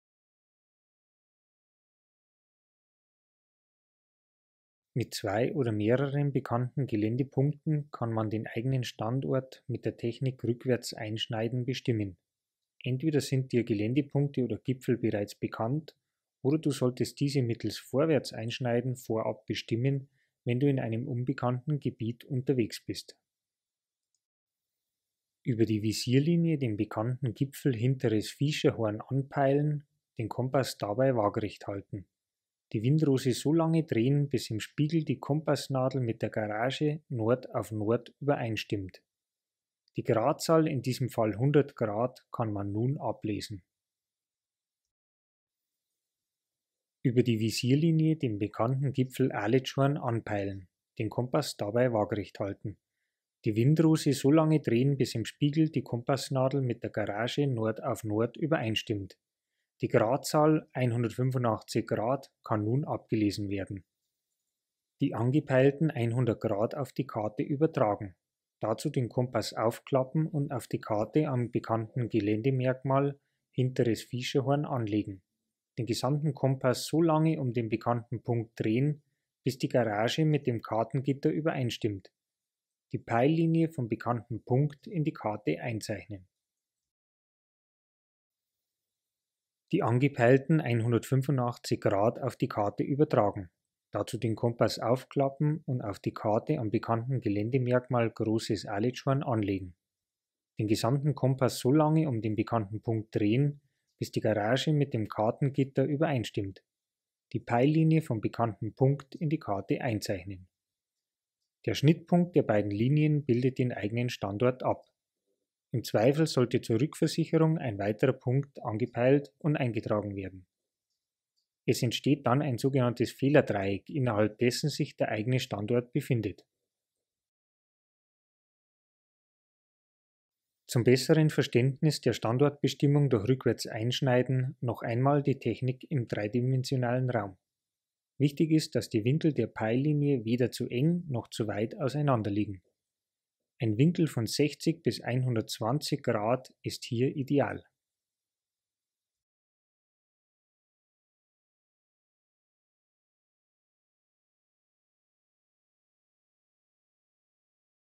#3 Rückwärts Einschneiden
Standortbestimmung mit dem Kompass durch rückwärts einschneiden. Mit dieser Technik können zwei oder mehrere bekannte Ziele angepeilt werden und so der eigene Standort bestimmt werden.